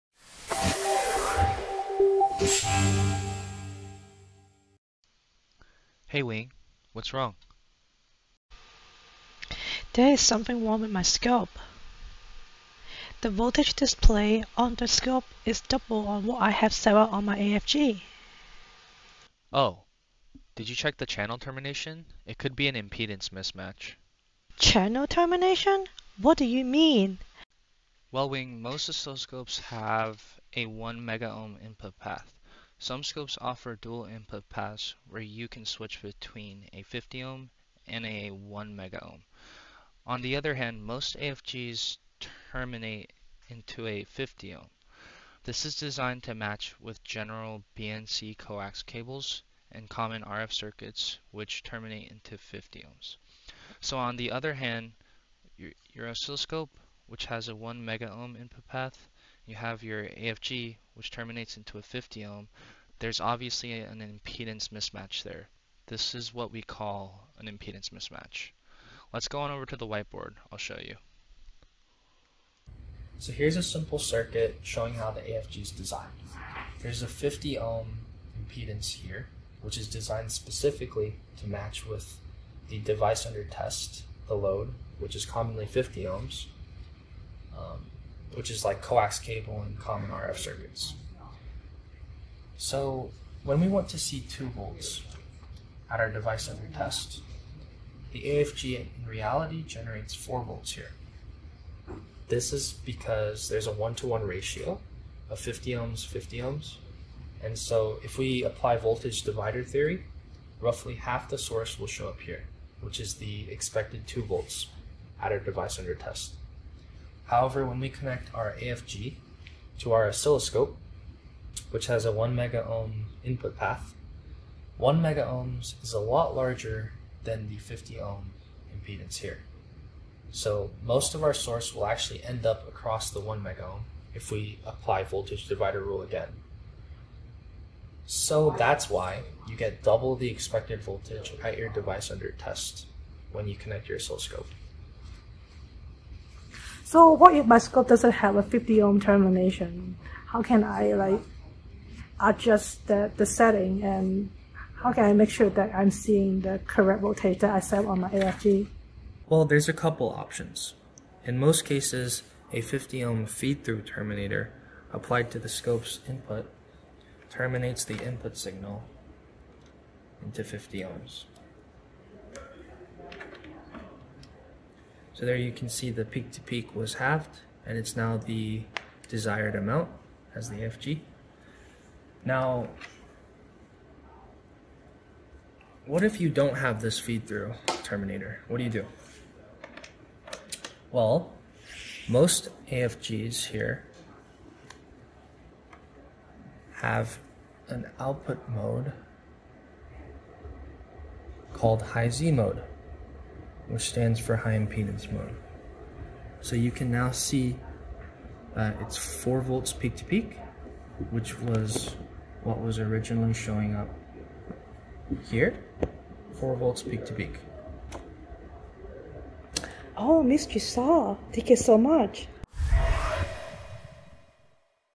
Resolving Impedance Mismatch
Learn more about impedance mismatch, which can cause the voltage that is displaying on the oscilloscope to be different than on the AFG.